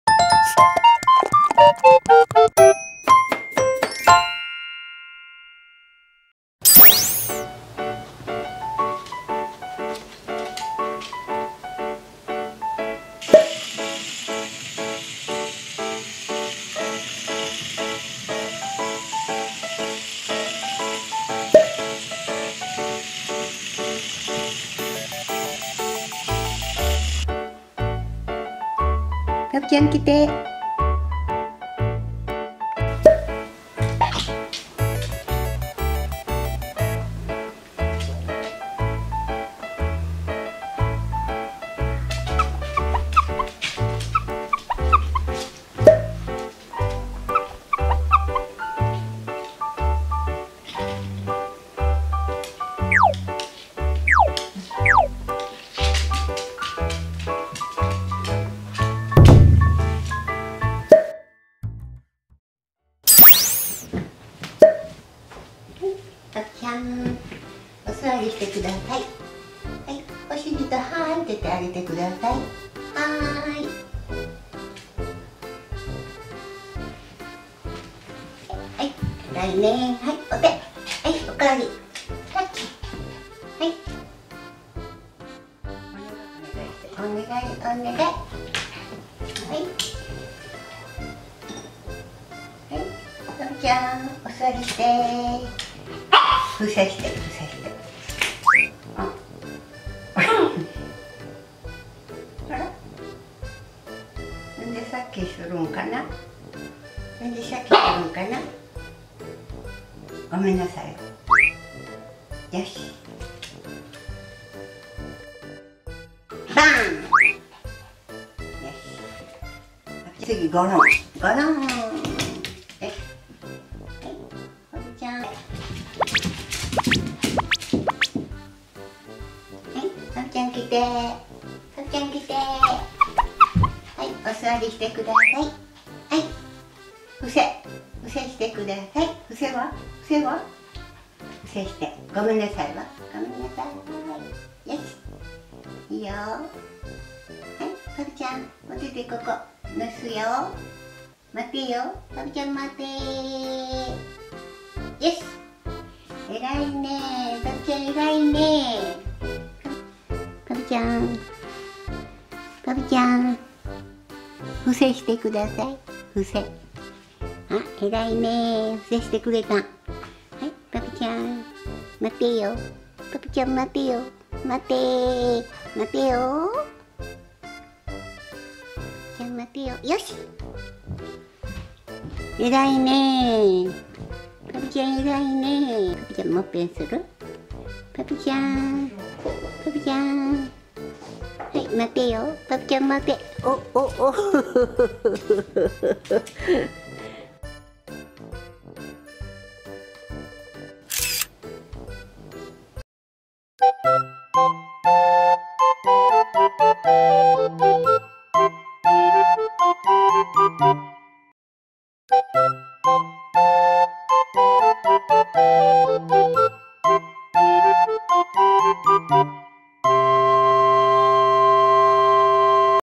おやつの時のルーティン【 犬芸 】
いつものように、勝手に進めていくパピーさんですが…
おやつを貰うのもひと苦労です　(;^_^A